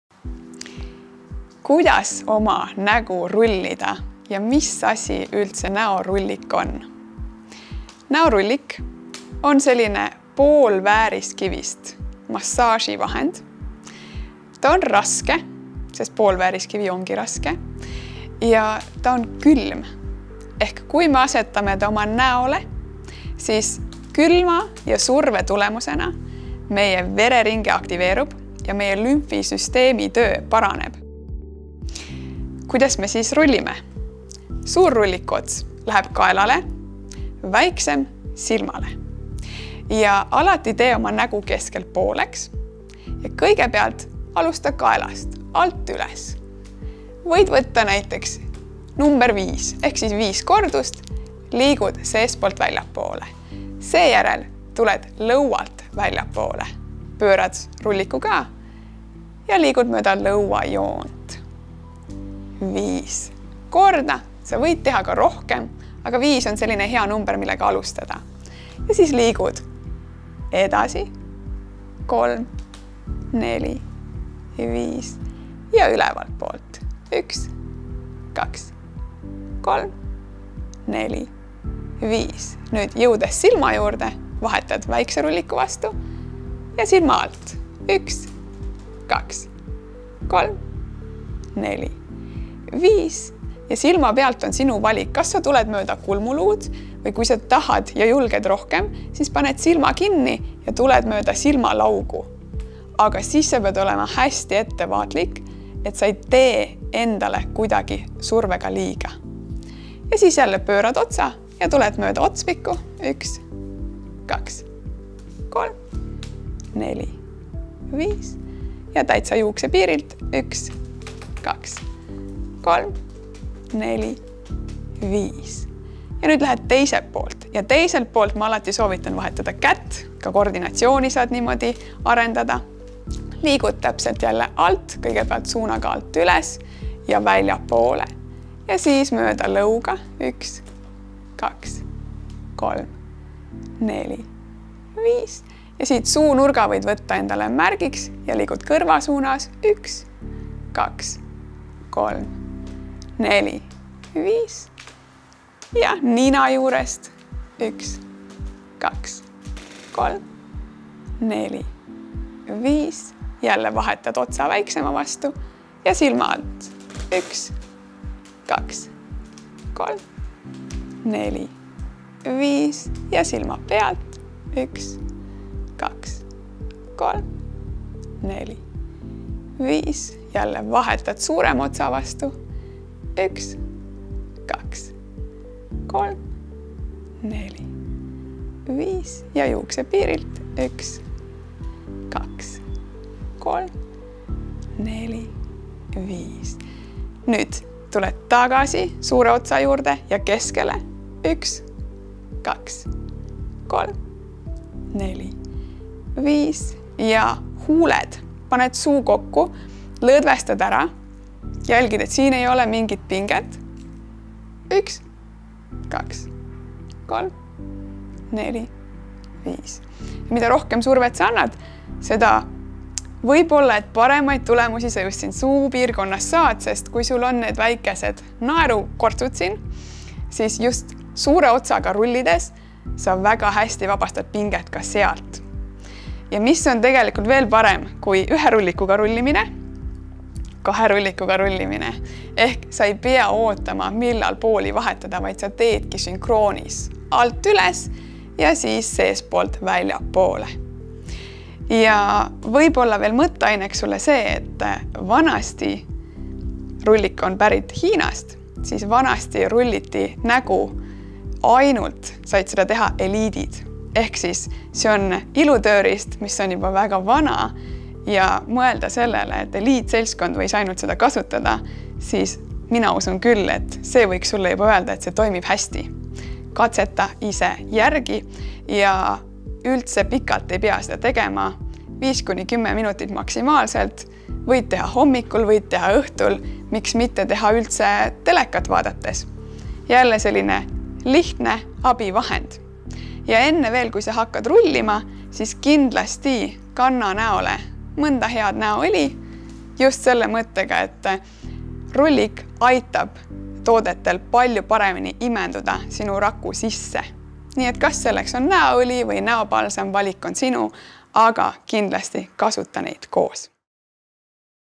Kuidas rullida oma nägu ja mis on näorullik?
MaiWistiku looja, joogatreener ja loodusterapeut Mai-Liis Kivistik selgitab, et näorullik on poolvääriskivist massaaživahend. See on raske ja külm ning näole asetades külma ja surve tulemusel vereringe aktiveerub ja lümfisüsteemi töö paraneb.

Vaata videot, kuidas näorullikut kasutada!

Abivahendid!